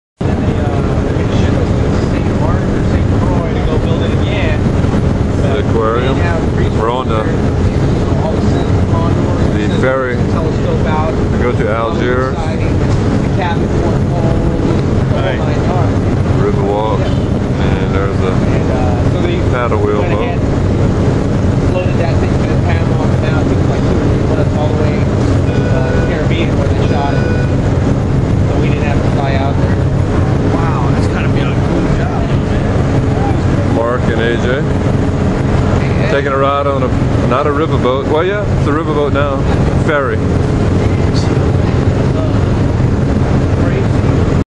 July 4,2010 New Orleans Ferry to Algiers from Canal St.
Fish(Mark)AJ,and Johnny going to historic 'Old Point Bar' (Algiers) New Orleans, LA